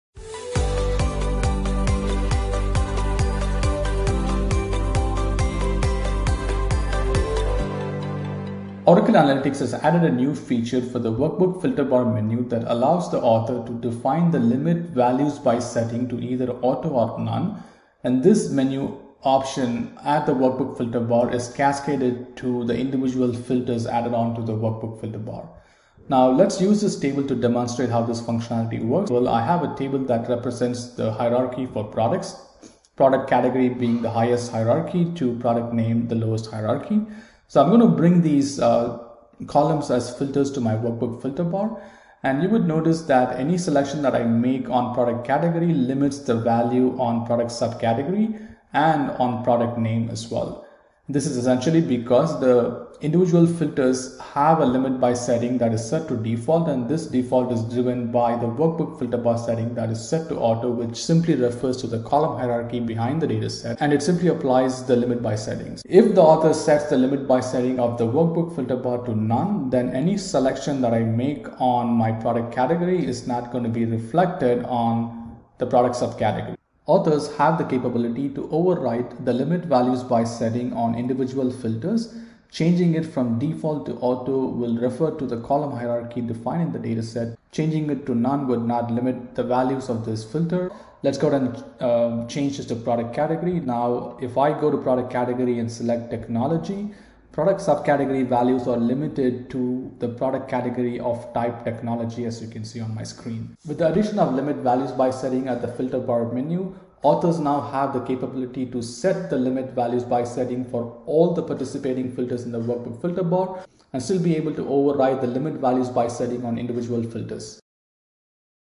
Cross-Filter Value Limits in Oracle Analytics Canvas
This video shows how to set default value for cross-filter behavior : what value can be seen in a filter when another filter has a value selected. This capability is part of the Oracle Analytics Nov 2022 update. A playlist of more of the new features coming with Nov 2022 update is available